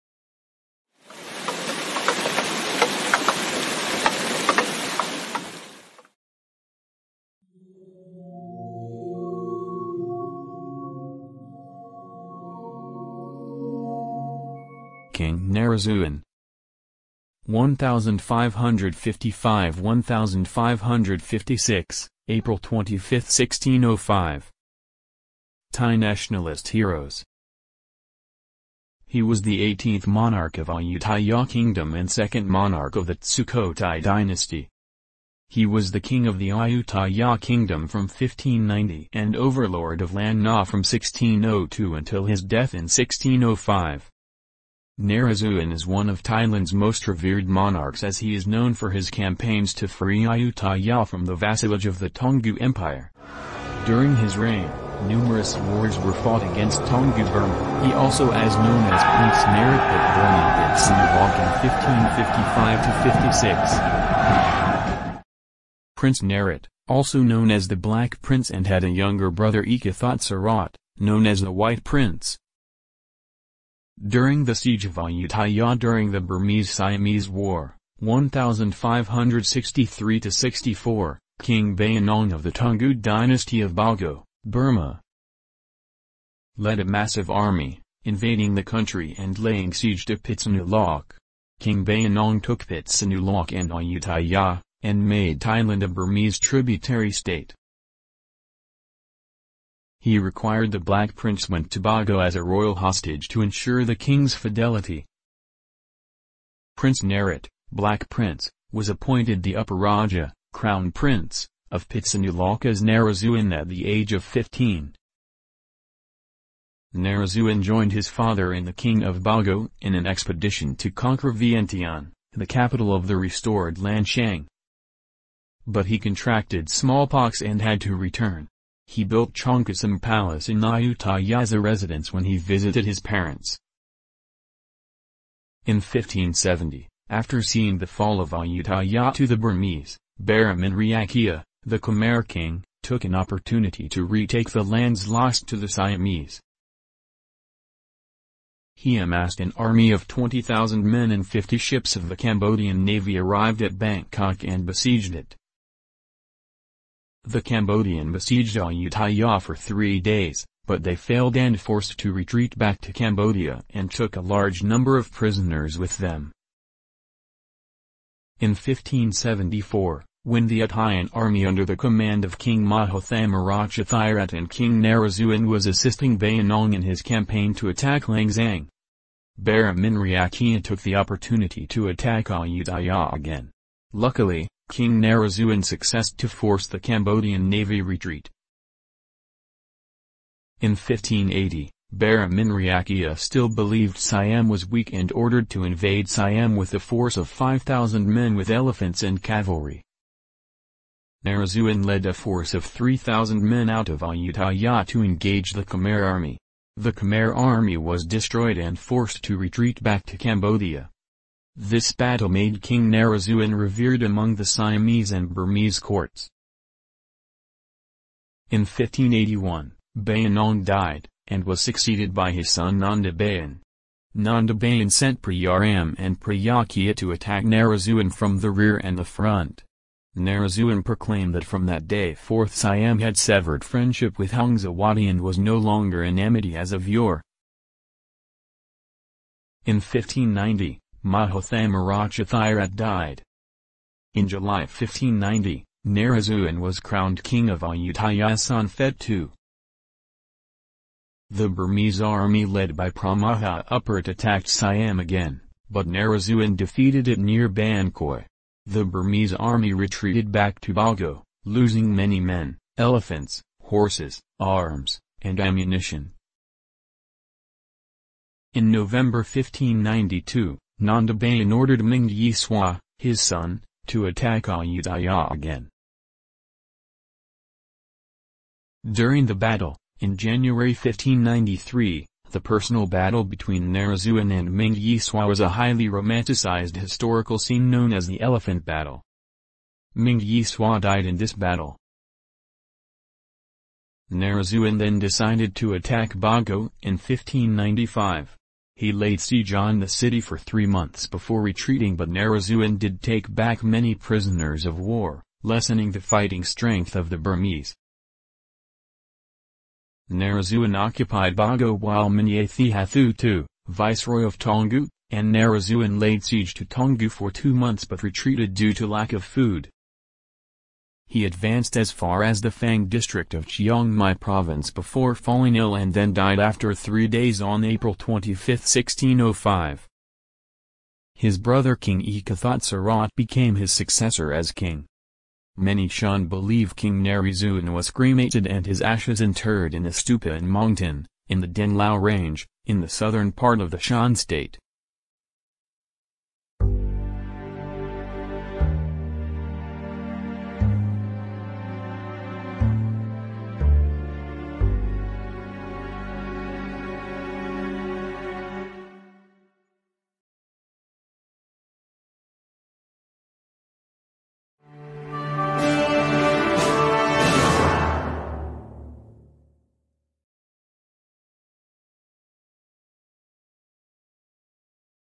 A HERO STORY FROM THAILAND
A story that tells of the might and courage of a king, to defend and protect the country and its people